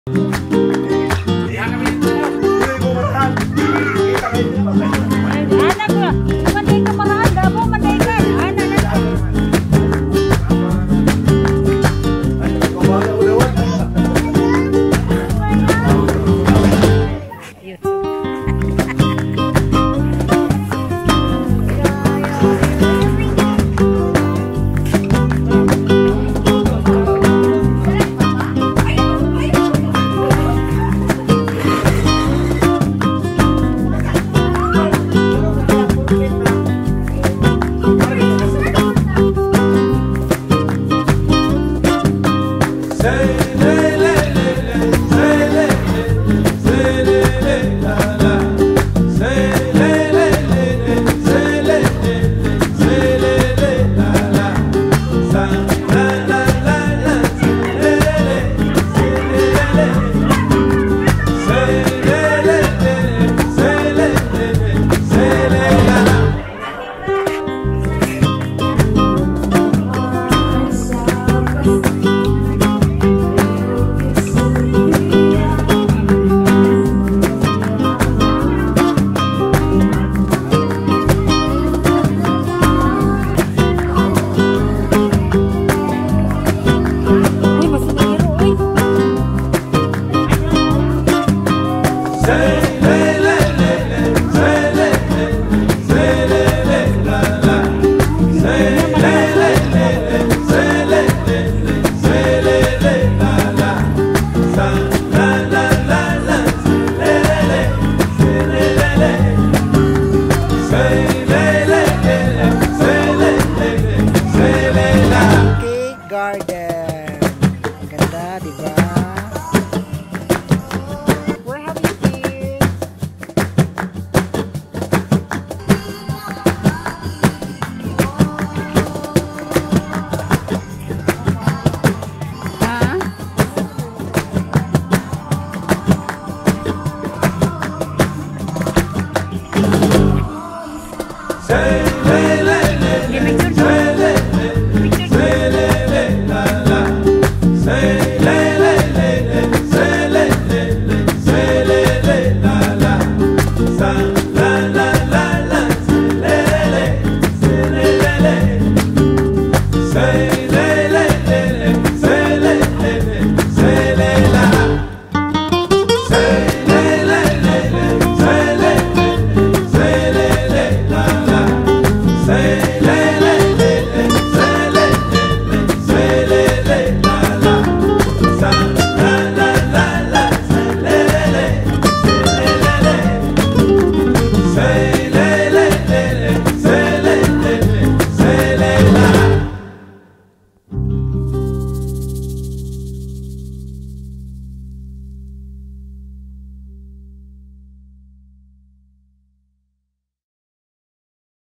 Province of Cotabato, Philippines
Quick visit to the Province of Catabato, Philippines
Strawberry Land Purok 4, Datu Ladayon, Arakan, Cotabato

Equipment I use:
Main Camera: Canon EOS 80D
Mavic Air 2
DJI Osmo Mobile 3
DJI Osmo Pocket
DJI Osmo Action
Mini selfie stick tripod kit